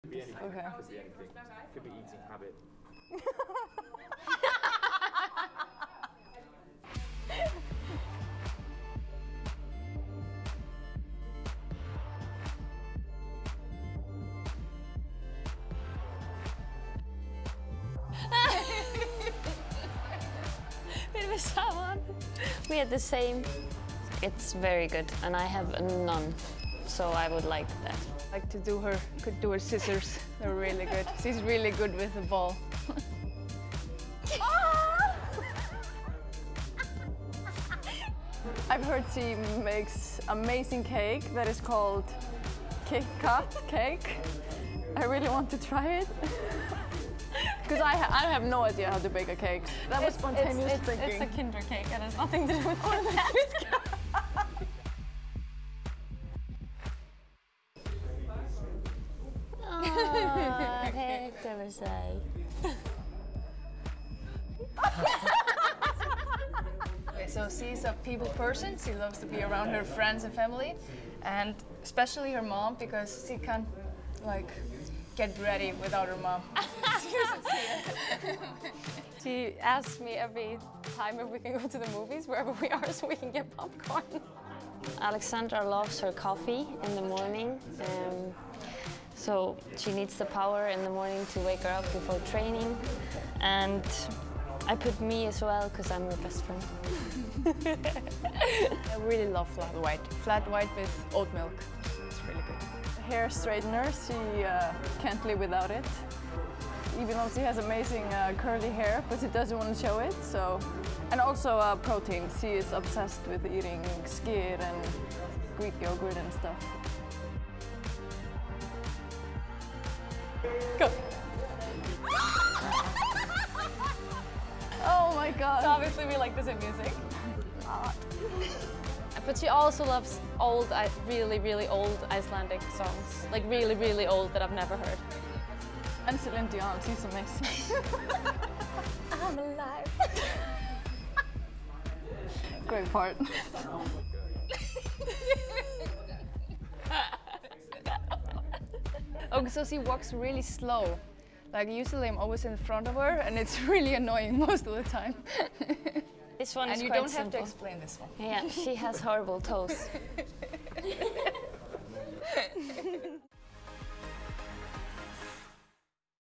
ICELAND Team-mates: VILHJÁLMSDÓTTIR & JÓHANNSDÓTTIR, THORVALDSDÓTTIR & VIGGÓSDÓTTIR | WEURO 2022
Describe the best characteristic in your team-mate.

What can't your team-mate live without?

A characteristic you would not like to take from your team-mate.

Iceland team-mates Karólína Lea Vilhjálmsdóttir, Alexandra Jóhannsdóttir, Berglind Thorvaldsdóttir and Glódís Viggósdóttir play our teammates game.